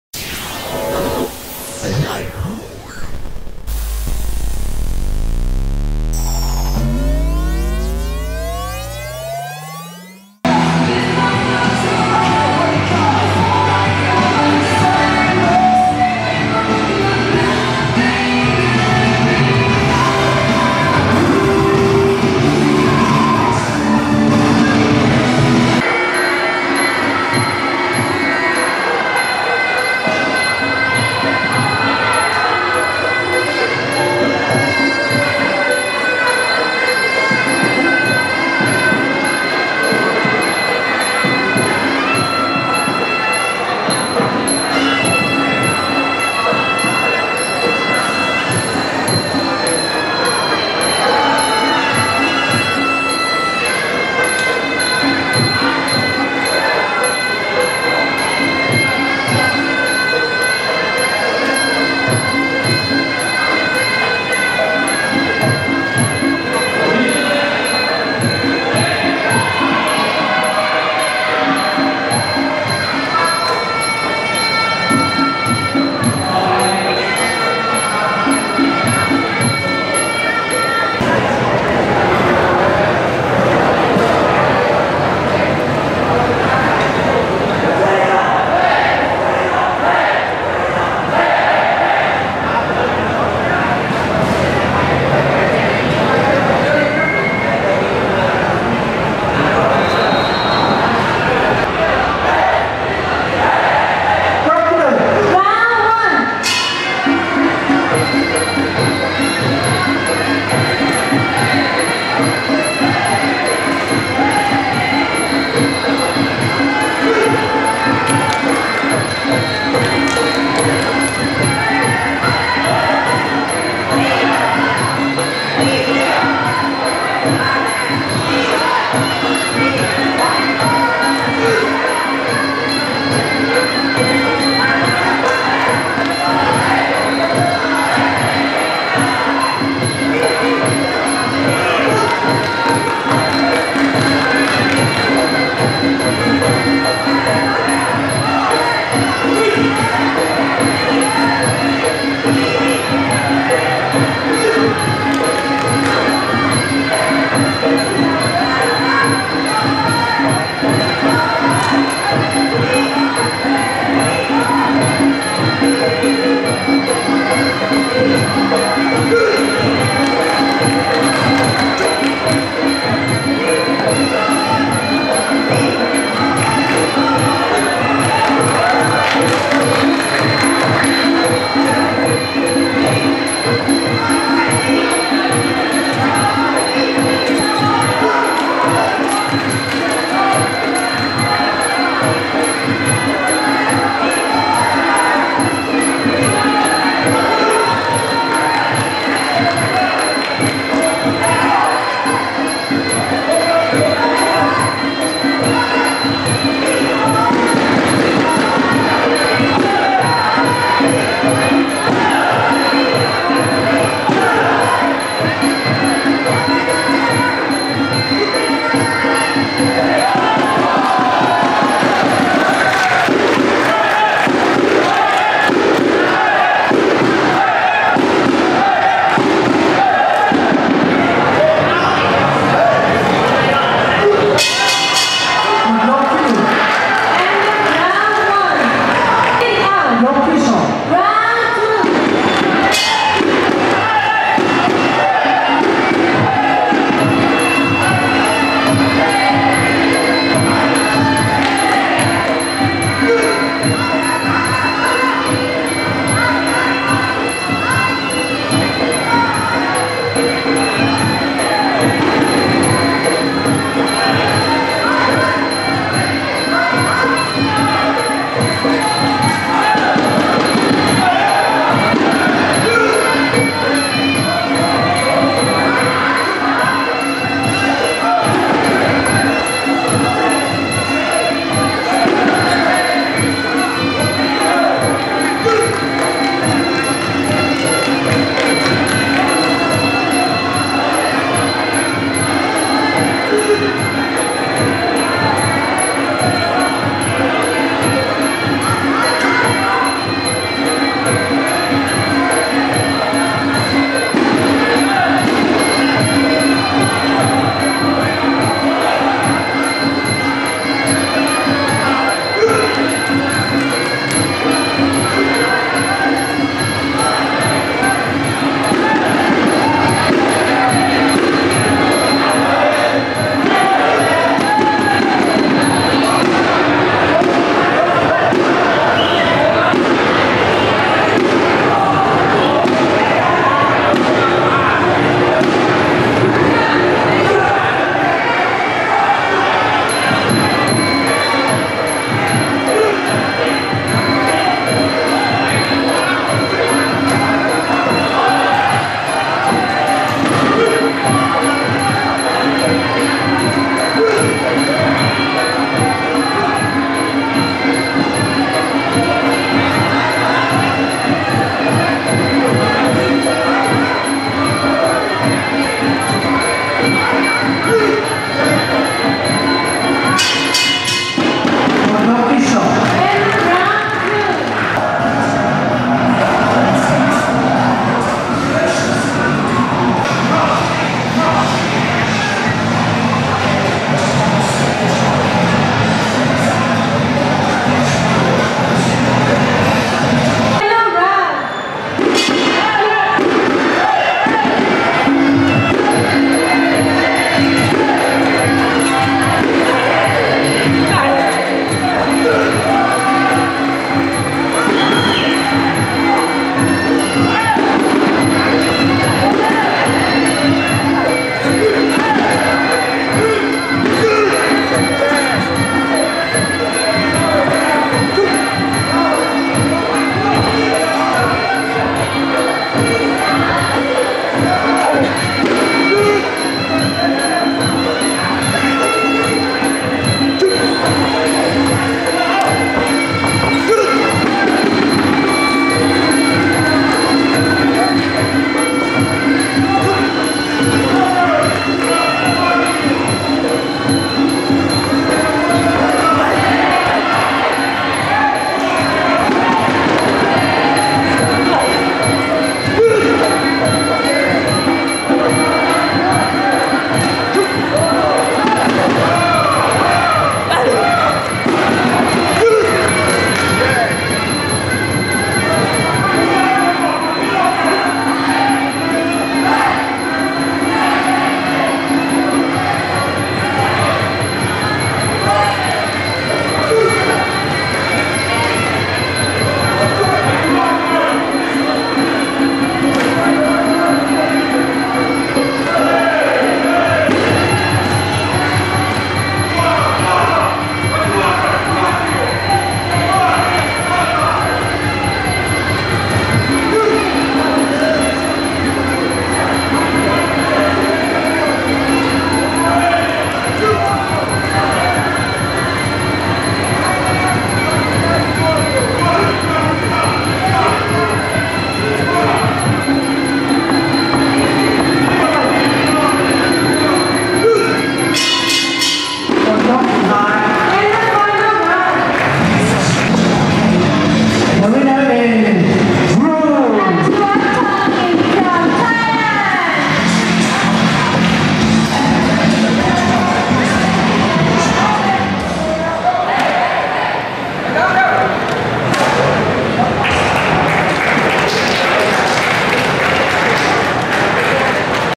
ЖЕЛЕВ ДАНИИЛ (Украина) - TONG-AEM ATHIWAT (Таиланд) финал
Чемпионат Мира по таиландскому боксу среди молодежи
IFMA Youth World Championship 2016